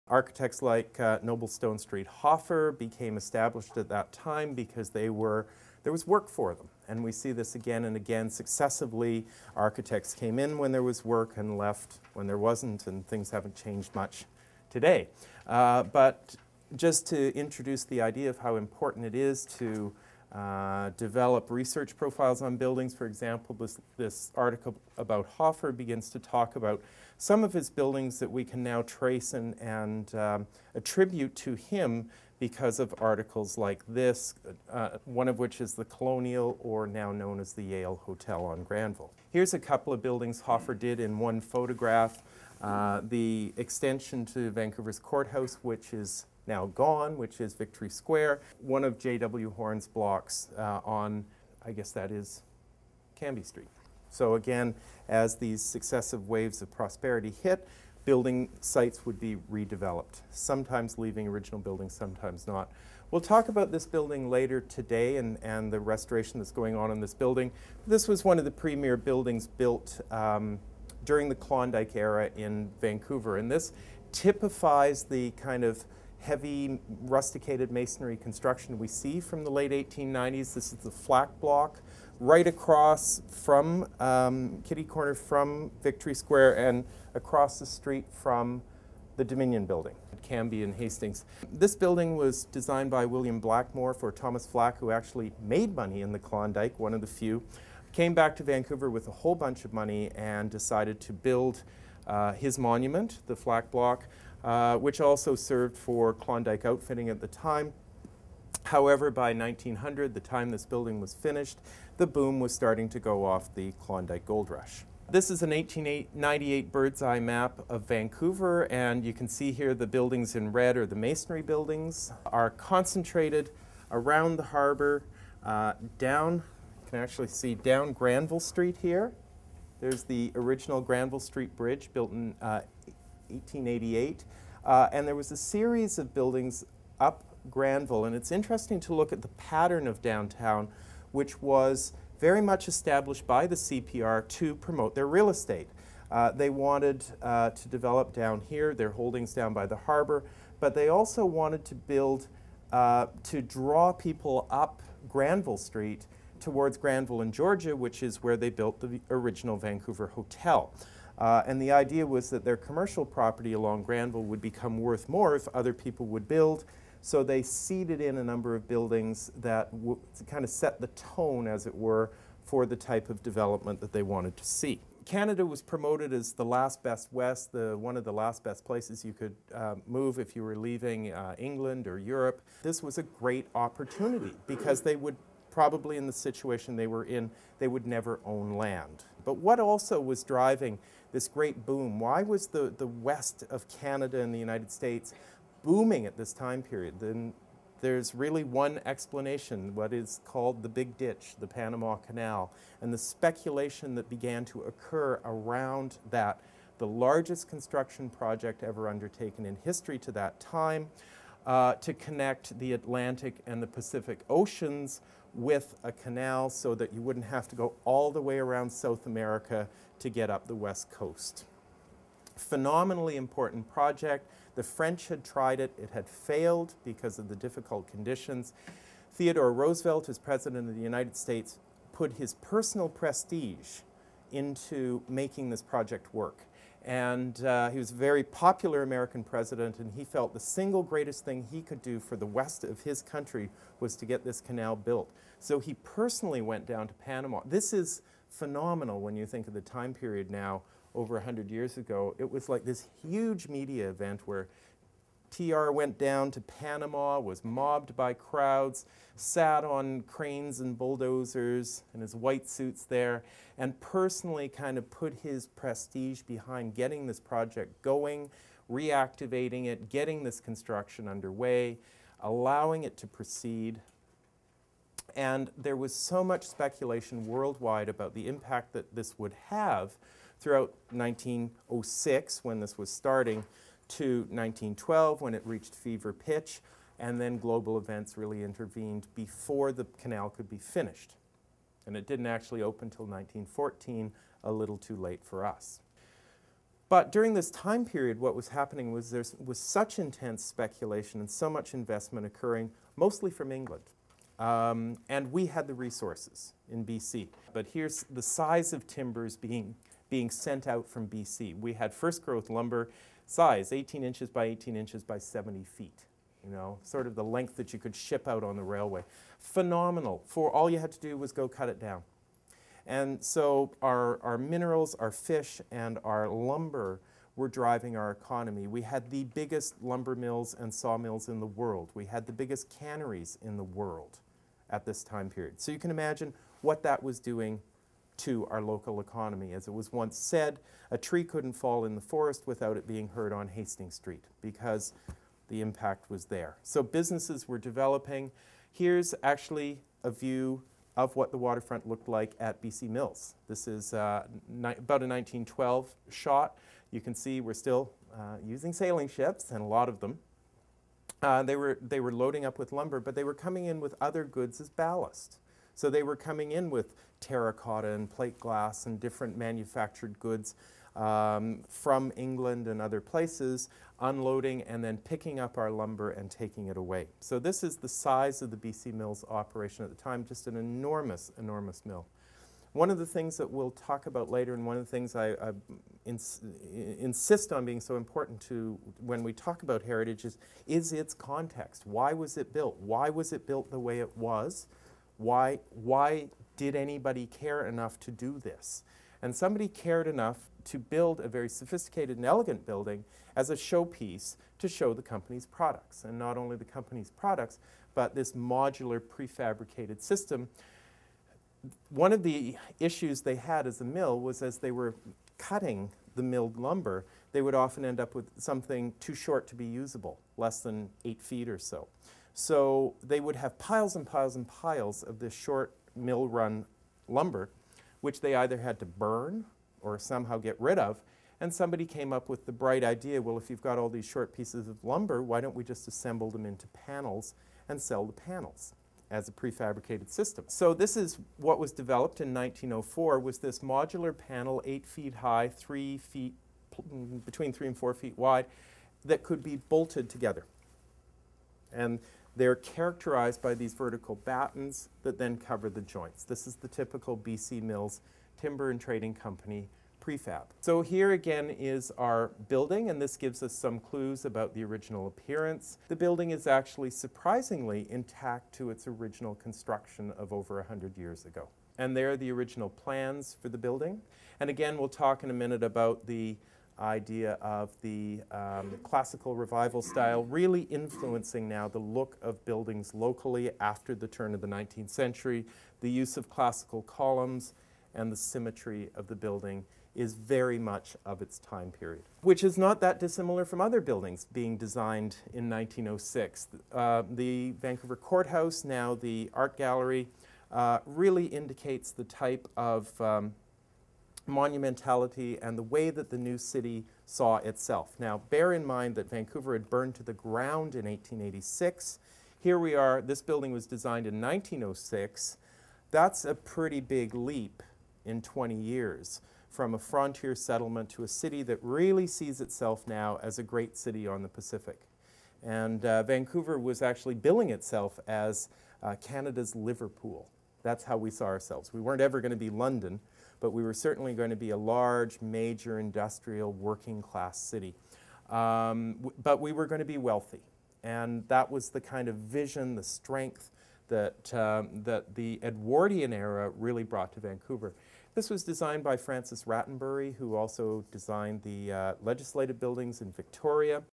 An Overview of Vancouver's Architecture by Donald Luxton PART2
Old School: Courses for Building Conservation
A sample lecture from the Vancouver Heritage Foundation's full day course "Heritage 101: Understanding Heritage Conservation", this is part 2 of 3 parts (10 minutes each) from An Overview of Vancouver's Architecture presented by Heritage Consultant Donald Luxton of Donald Luxton & Associates. Some of the topics discussed are: The forces of history in Vancouver—economics, politics, social trends + environmental considerations, Evolution of building design—role of technology; use of different building materials, Vancouvers architecture as an expression of building the West, What are the values of existing buildings and their sites? How has this changed over time?